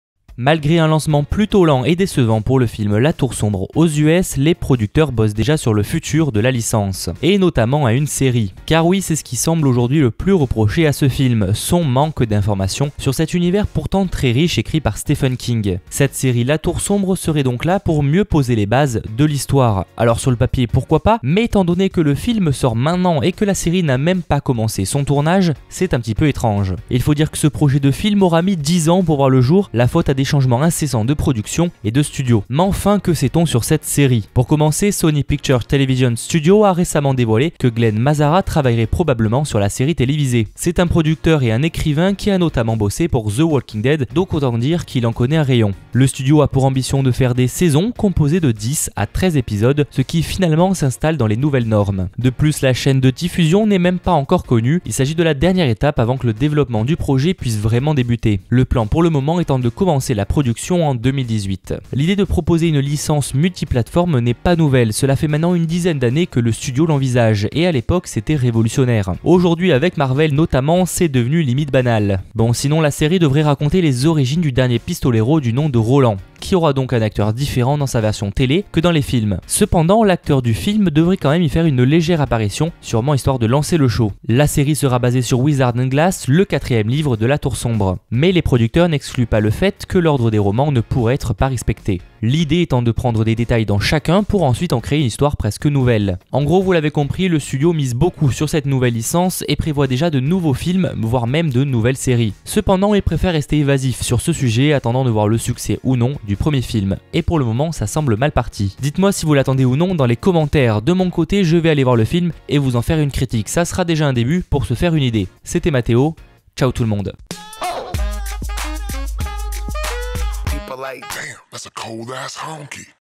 Le film La Tour Sombre se décline dans une série
On aura bien le droit à une série sur l'univers écrit par Stephen King, et on a déjà de nombreuses informations sur le sujet !

Pourquoi ne pas prolonger l'expérience geek en visitant notre site internet ?